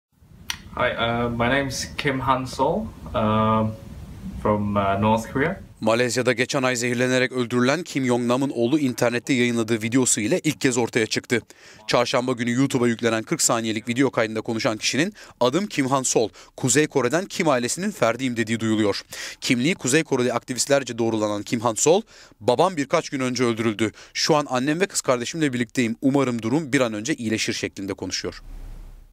Kuzey Kore'nin kayıp varisi Kim Han Sol Youtube'da ortaya çıktı
Malezya’da geçen ay zehirlenerek öldürülen Kim Jong-nam’ın oğlu Kim Han Sol kendini dünyaya ilk kez bir youtube videosu ile gösterdi.

Videoda pasaportunu da gösteren Kim Han Sol'un videosu Cheollima adlı bir grup tarafından Çarşamba günü Youtube’a yüklendi ve tüm dünya medyasında manşet oldu. 40 saniyelik video kaydında konuşan kişinin kimliği Kuzey Koreli aktivistler tarafından doğrulandı.

Videoda "Adım Kim Han-sol, Kuzey Kore’den, Kim ailesinin ferdiyim" diyen Sol babasının birkaç gün önce…